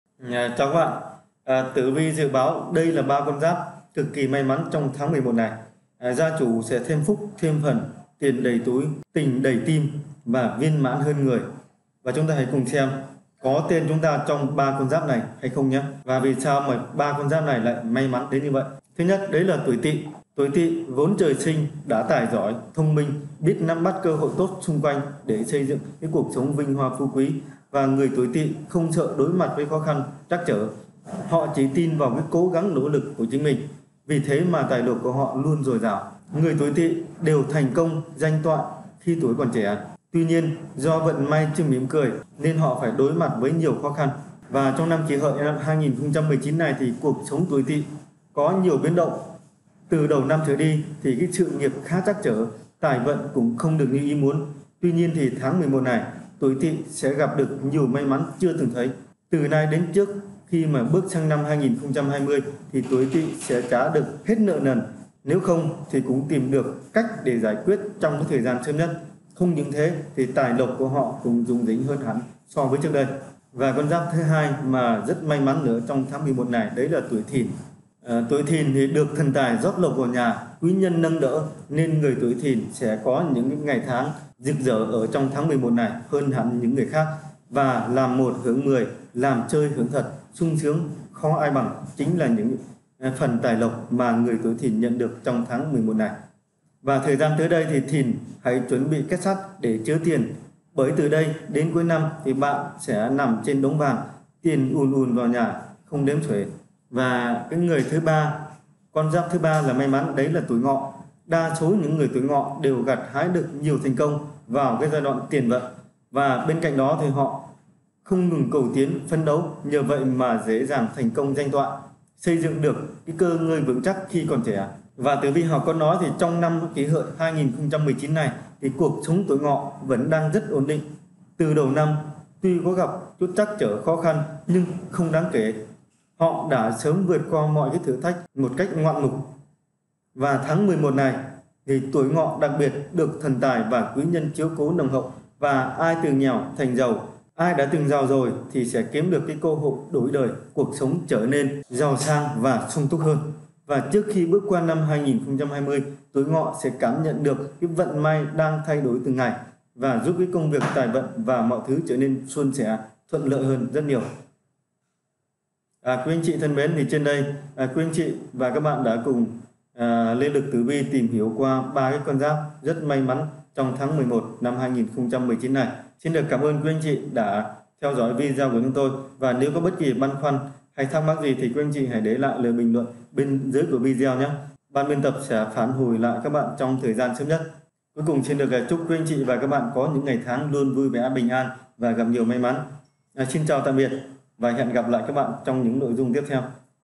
3 con giáp làm đâu may đấy, tiền tài "rủ nhau" đến gõ cửa trong tháng 11/2019 | Lê Lực Tử Vi
3 con giáp làm đâu may đấy, tiền tài "rủ nhau" đến gõ cửa trong tháng 11/2019 | Lê Lực Tử Vi
➤Xem thêm: